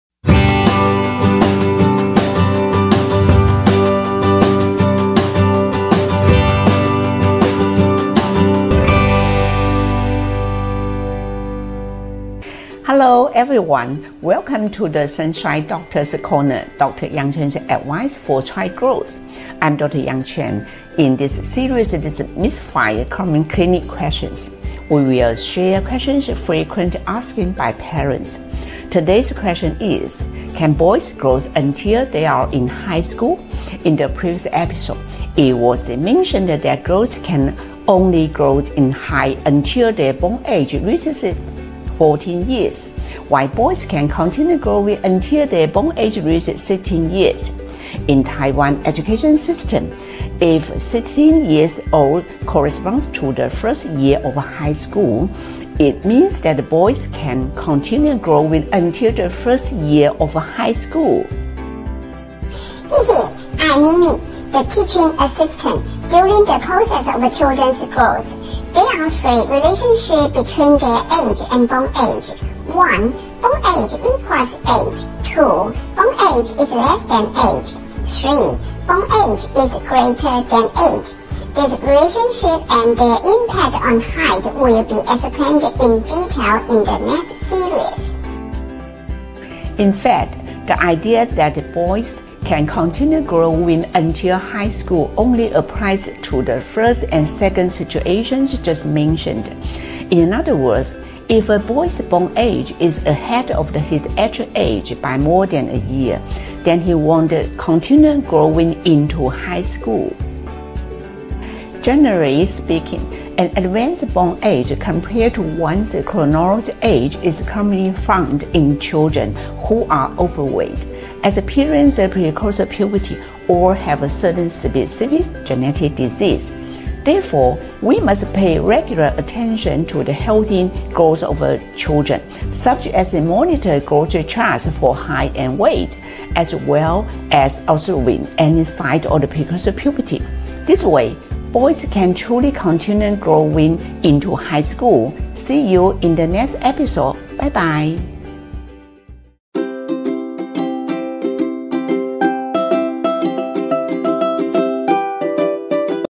【Demystifying Common Clinic Questions 3】Can boys grow until they are in high school?
🖥️ In the new program " Demystifying Common Clinic Questions," we will share six questions commonly asked by parents, letting everyone know how to further achieve children's healthy growth using the concept of " Demystifying Common Clinic Questions." Today's question is: Can boys grow until they are in high school? In the previous episode, it was mentioned that girls can only grow until their bone age is 14, while boys can grow until their bone age is 16. In Taiwan's educational system, if 16 years old corresponds to the first year of high school, in other words, boys can grow until the first year of high school.

With love,
Dr. Yang Chen